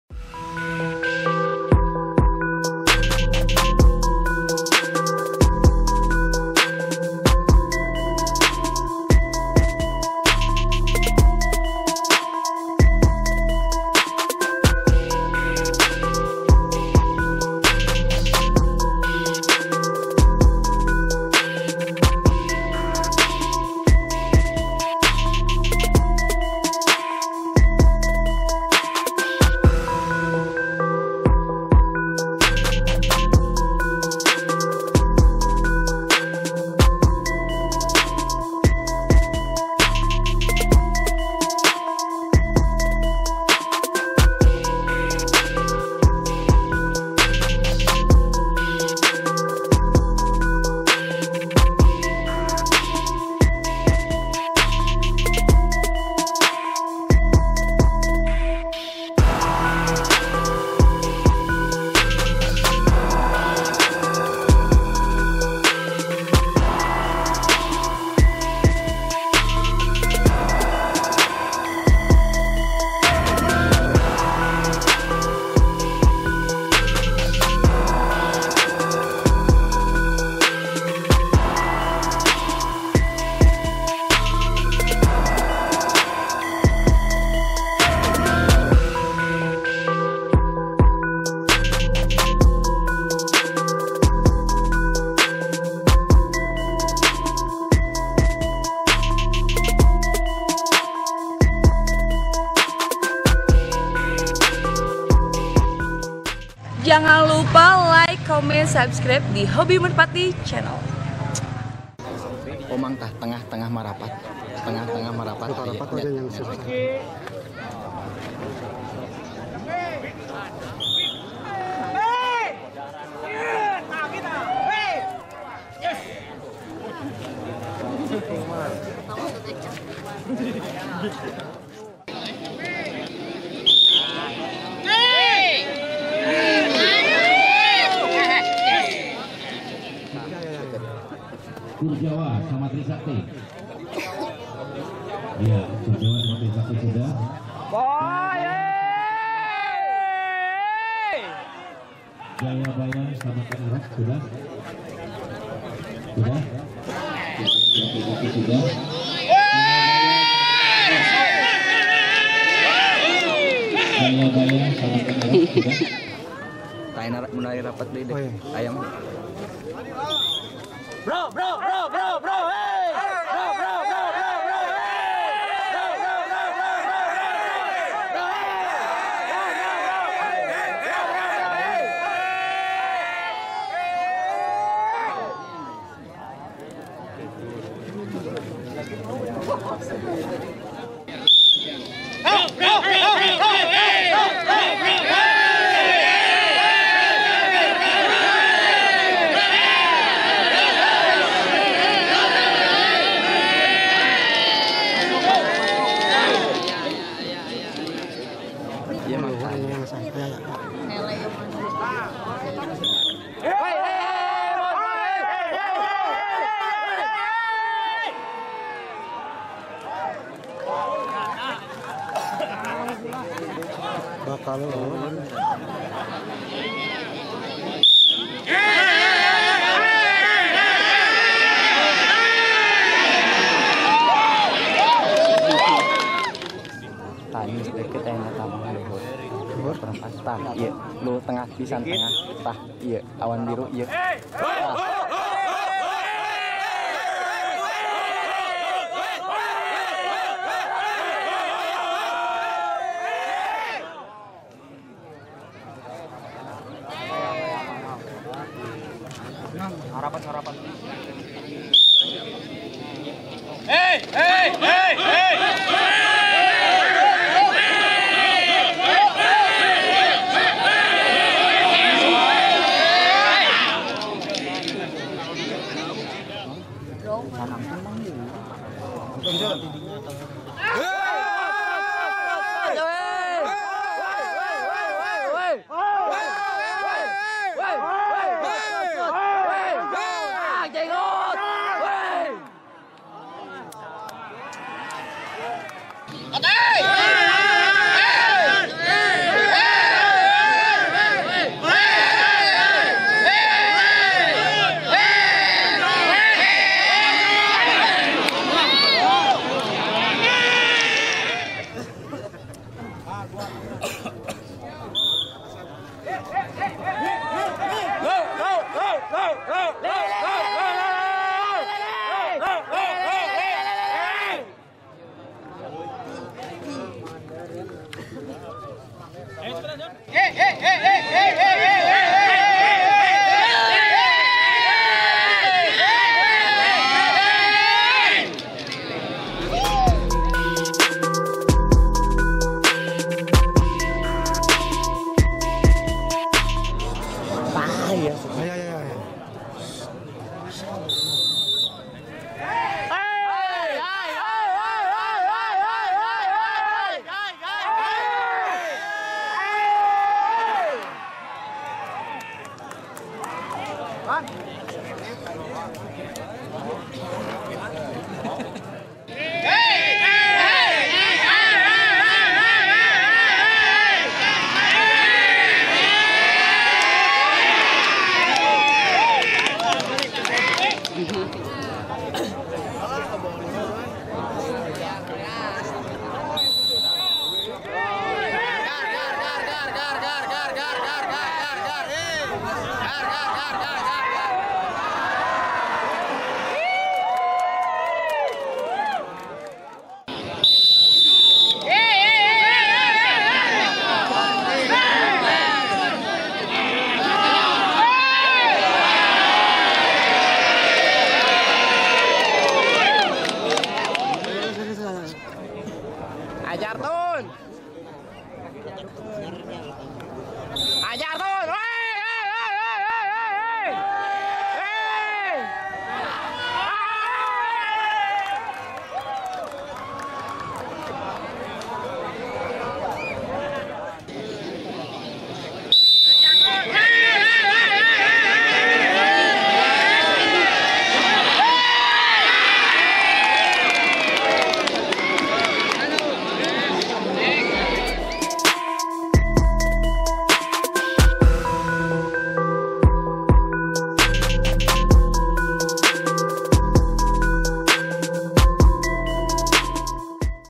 DETIK-DETIK JAYABAYA JUARA DI NASIONAL BANJARSARI (PART 2)
Lomba nasional Banjarsarai menjadi sejarah bagi jayabaya. Soalnya, butuh empat bulan jayabaya berada di tangan Royal hingga klimak juara satu. Sebelumnya Jayabaya sempat juara 2 di bilabong dan juara empat di lapak GM bandung. Di lapak Desa Banjarsari Jayabaya Klimak di poidum satu. Pertarungan mulai sengit dibabak kelima. Player-player yang telah memiliki poin tumbang. Simak vidonya